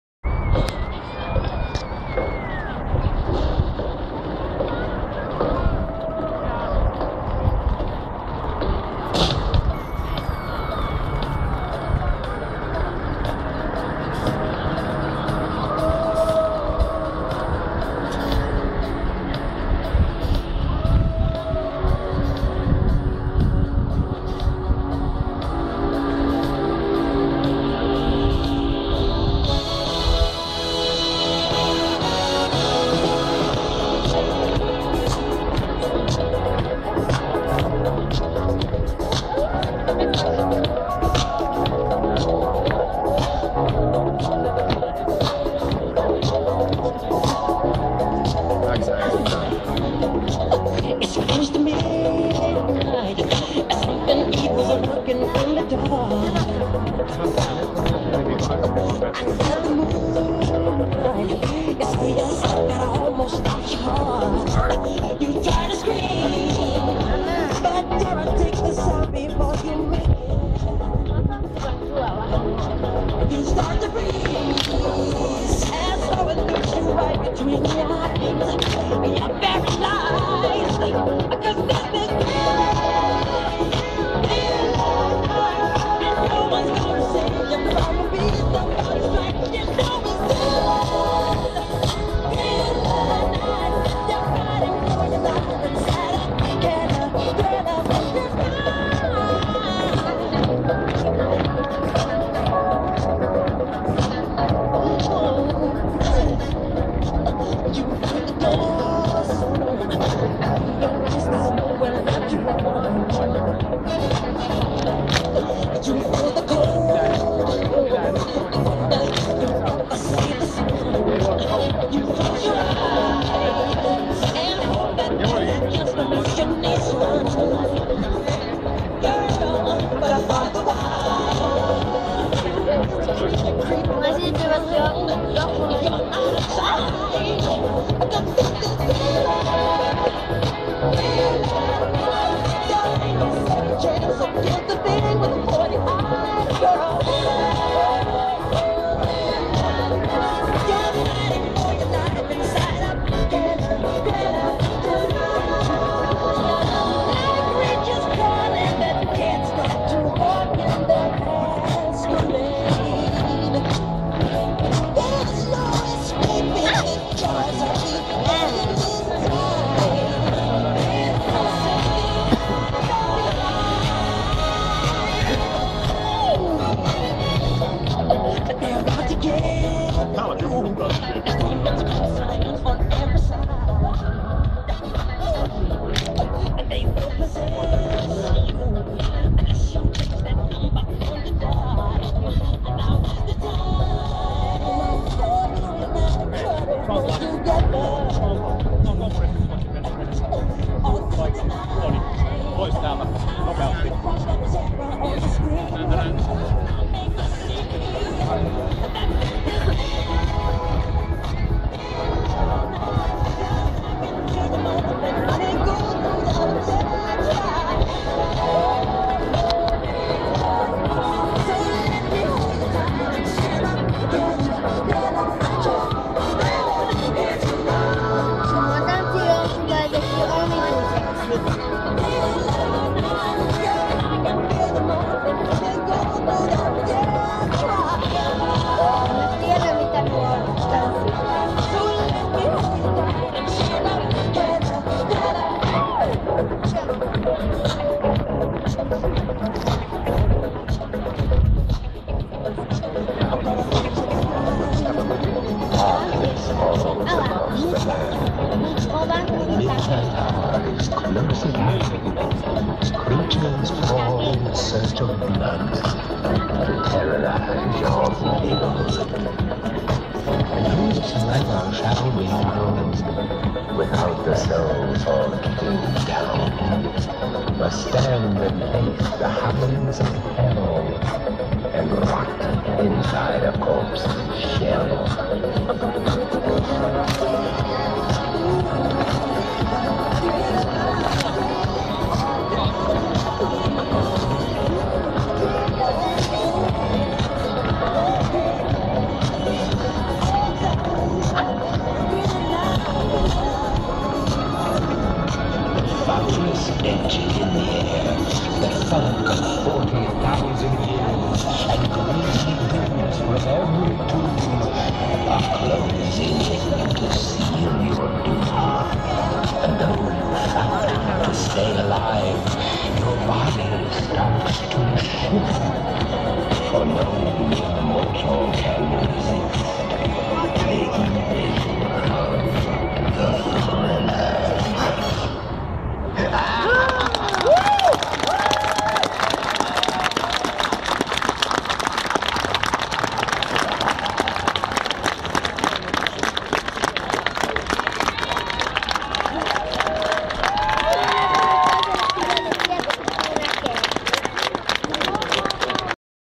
Thrill The World 2011 | Helsinki
Part of the worldwide dance performance 'Thrill The World'.
29th October 2011 5pm EET.